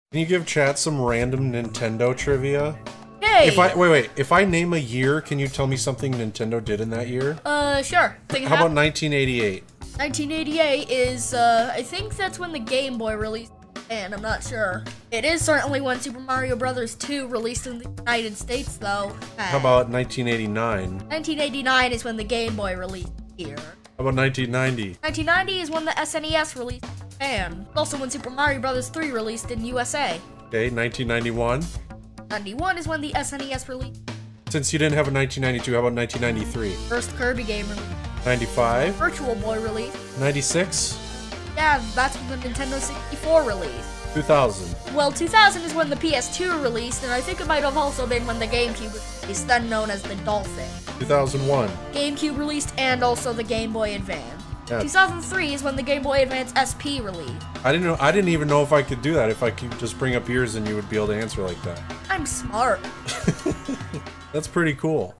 My Son, The Nintendo Historian
Being on the autism spectrum is a superpower, confirmed. I forgot where I put my car keys this morning but Roman will tell you what games released in 1995.